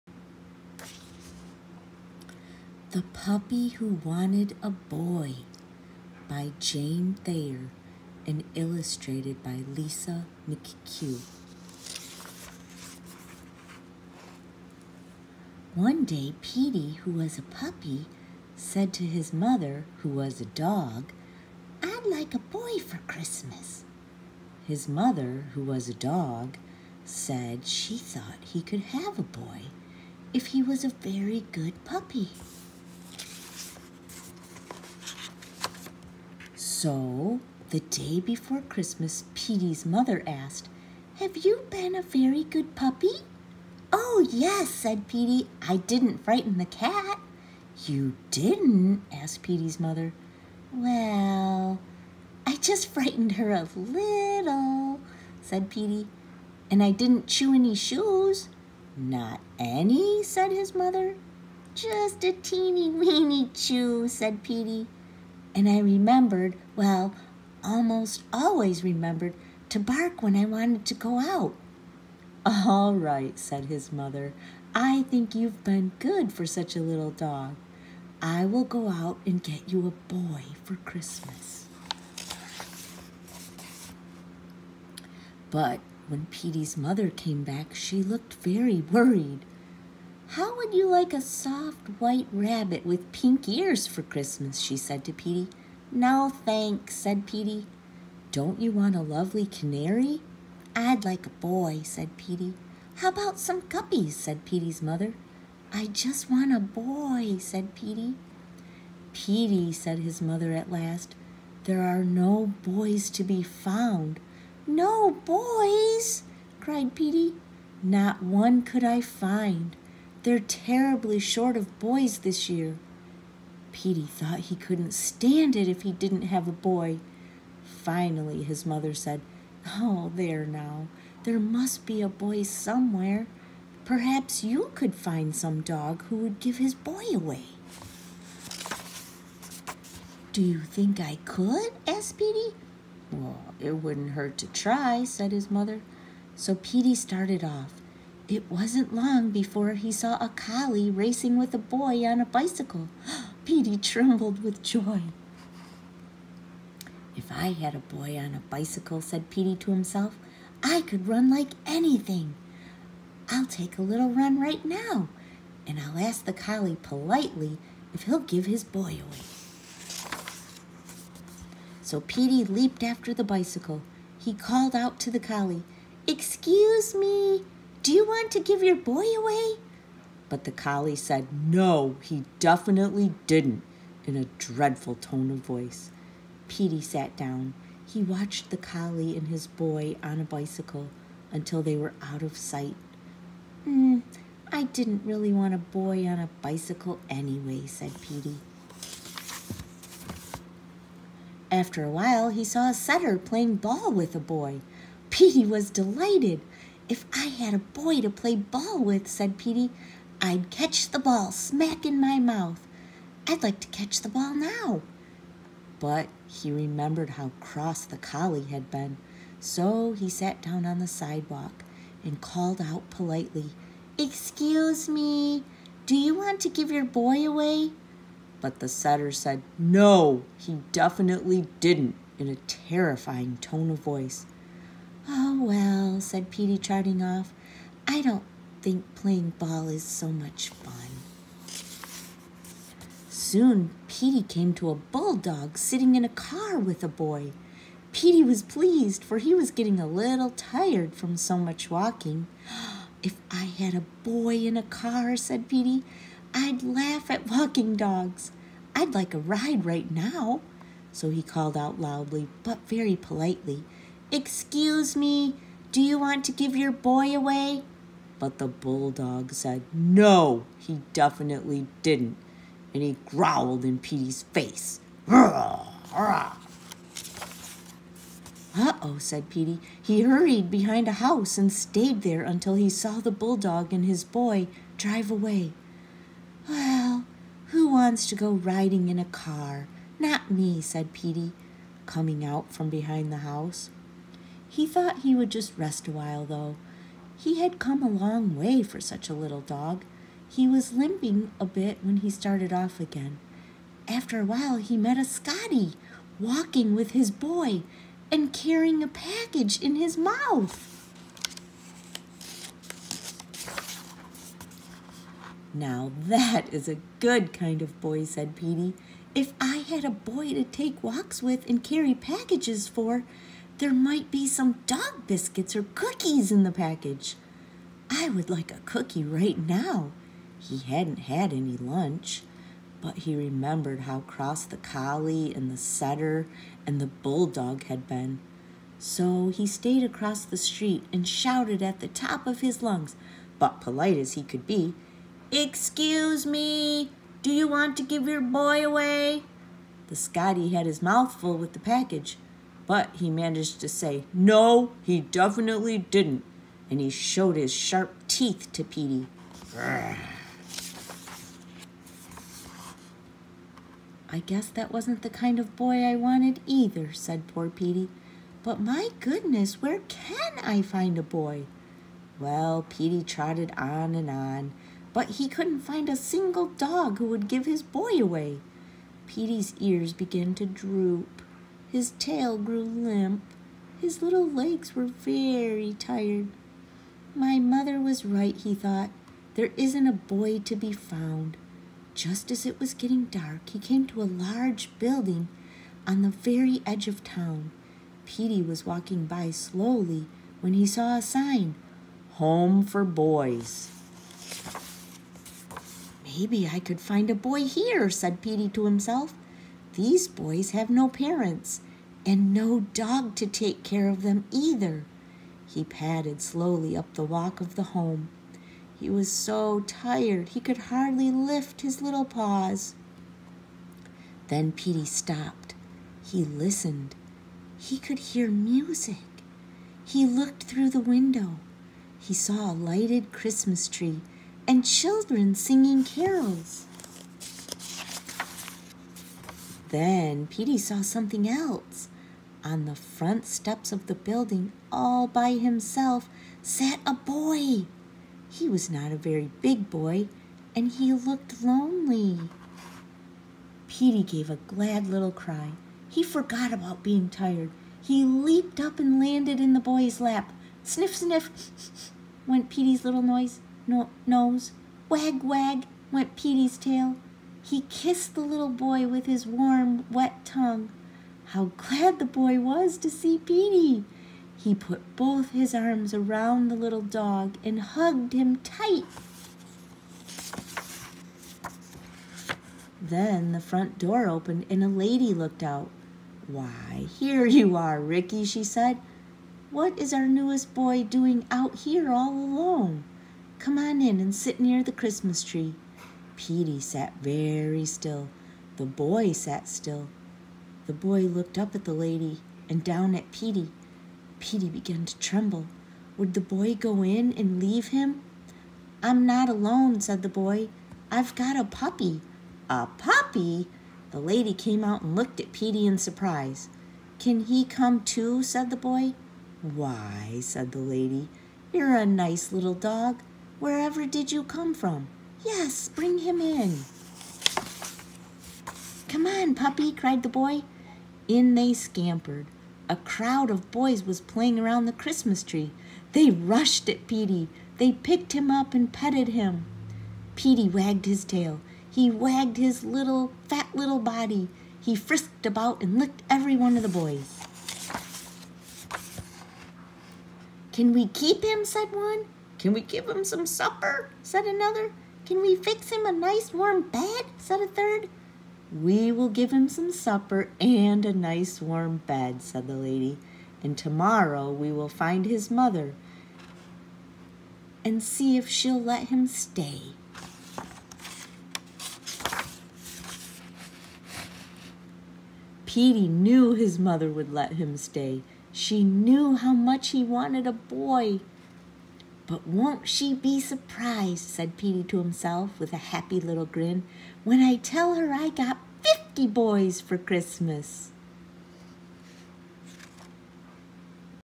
The Puppy Who Wanted A Boy by Jane Thayer and illustrated by Lisa McCue
Petey is a puppy who wants a boy for Christmas!

There’s a shortage of boys, though. Will Petey find one?